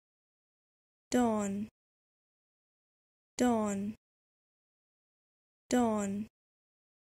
How to Pronounce Dawn
Learn how to pronounce Dawn correctly. This video shows you how to pronounce Dawn correctly in American English.